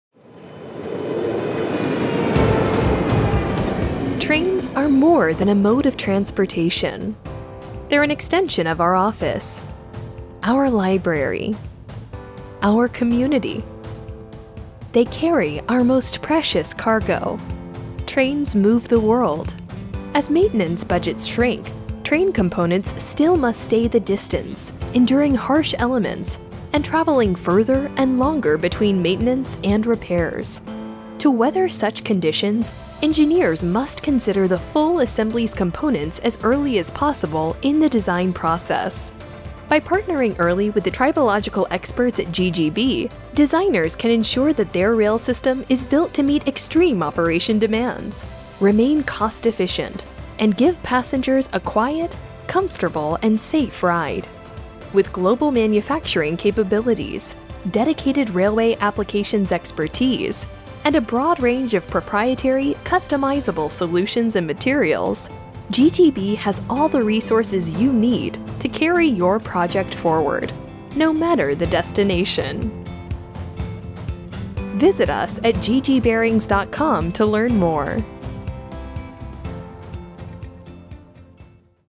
GGB Railroad & Train Bearings for Railway Applications | GGB Videos
As railway technology increases, railroad and tramway vehicles require more heavy-duty, high-performance bearings that can withstand harsh operating and environmental conditions. Our self-lubricating plain railway bearings offer high-load capacity, low friction and a large operating temperature range. These railroad bearings are ideal for helping to reduce noise and vibration, increase energy efficiency, and extend maintenance intervals.